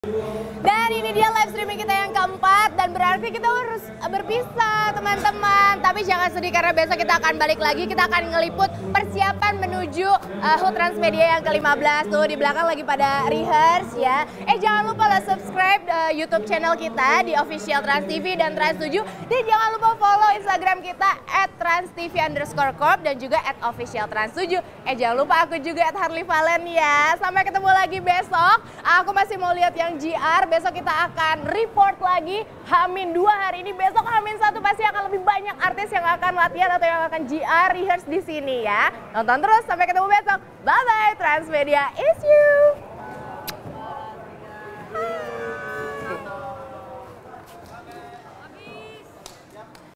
Persiapan Hut Transmedia 15 You Take 5
Yuk Kepoin Persiapan Hut Transmedia Bareng Harly Valentina Seru abiss !!